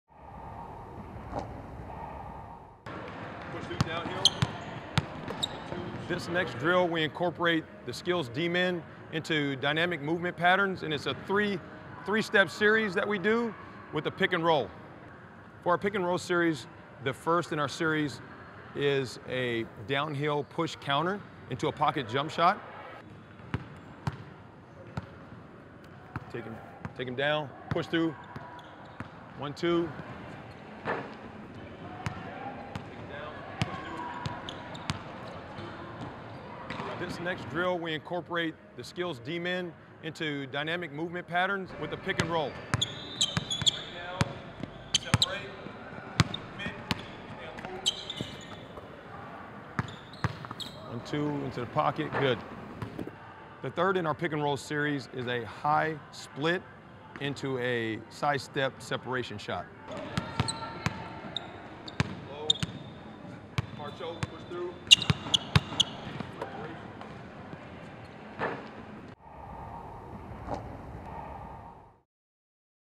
406535 D MAN PICK AND ROLL SERIES
Hone your offensive skills with the help of the SKLZ D-Man Defensive Basketball Guard. Perfect for use in both team and individual practice drills. It is ideal for practising passing and shooting around hands up defenders allowing for more creative team play around in the opposition half. Lightweight and portable, they are easy to set up thanks to the inclusion of a base and a ground stake to keep them stable. With an adjustable telescoping pole allowing for defender heights between 6.5' to 8'.